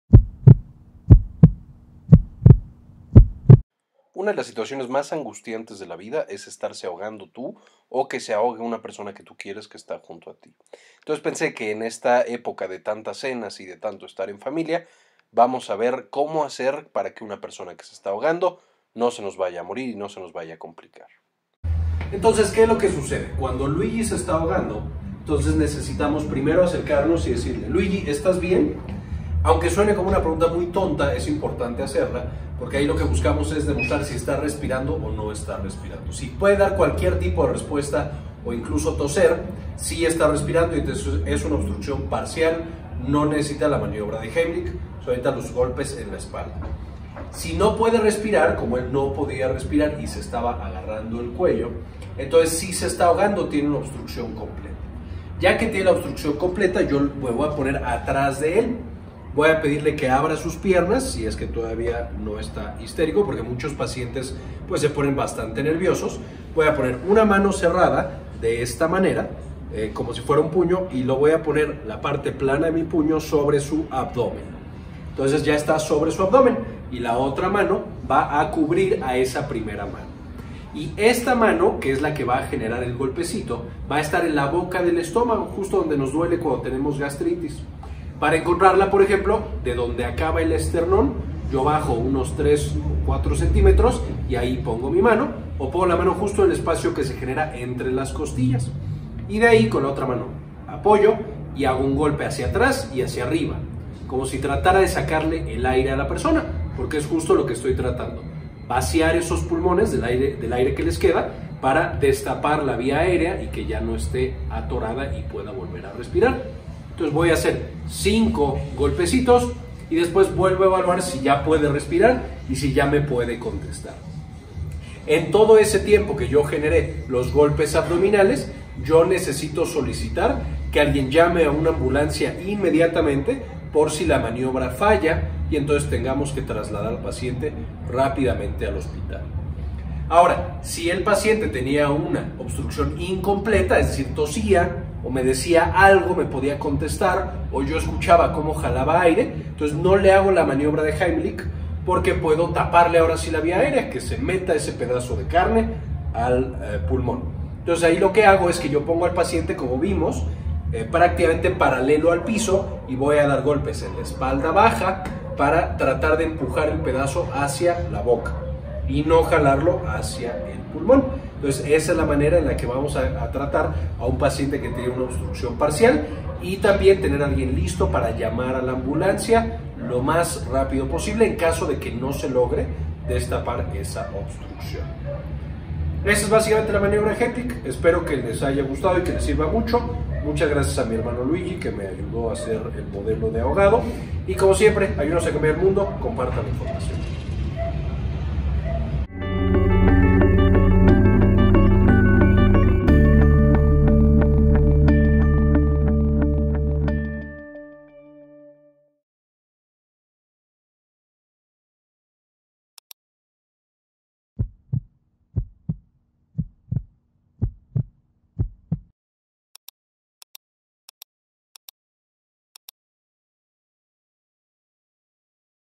¿Qué hacer si alguien se está ahogando?
Es aterrador cuando alguien que amamos o nosotros mismos nos empezamos a ahogar con un pedazo de comida, en este video veremos qué hacer.